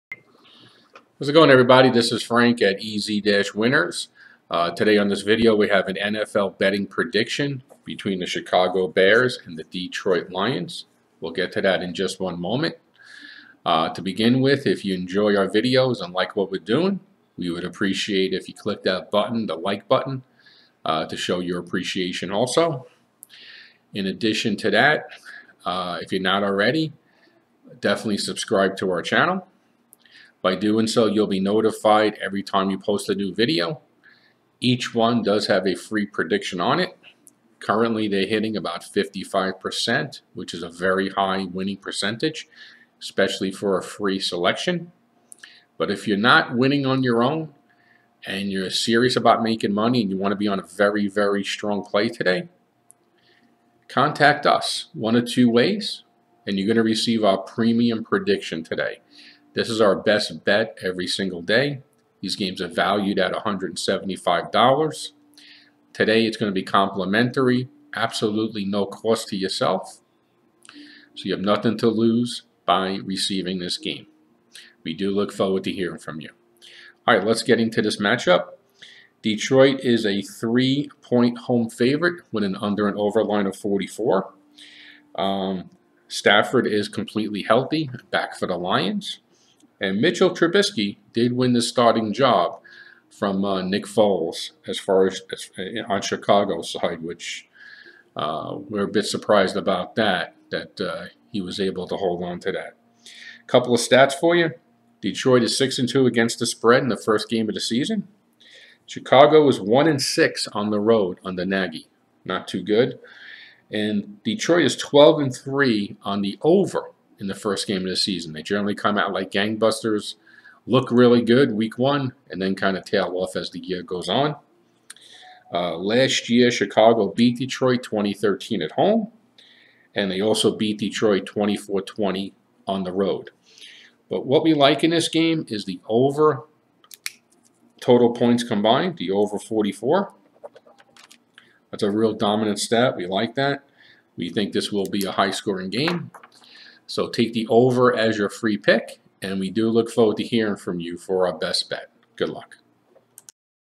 Bears vs Lions Picks NFL Week 1 Betting Predictions 9-13-20
If your NFL betting picks have you down today get a free week 1 pro football prediction tonight between the Chicago Bears vs Detroit Lions 9-13-20